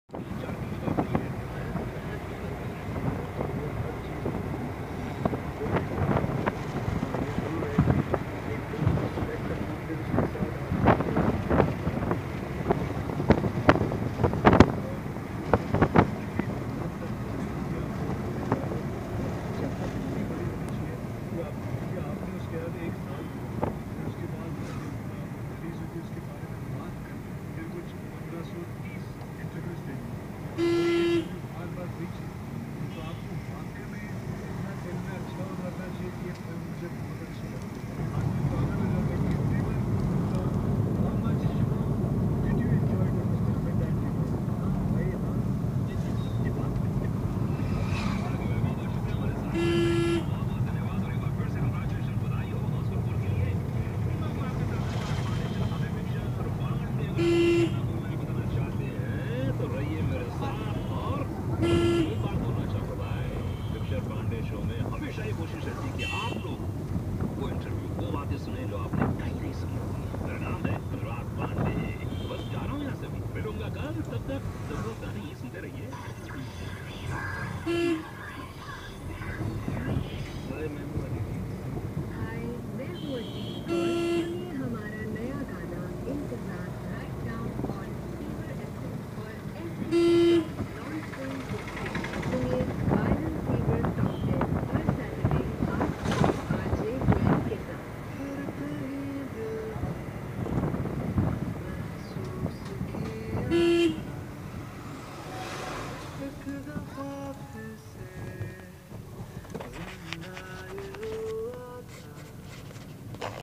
On the way to Mangalpur village
Meeting with villagers and Panchayat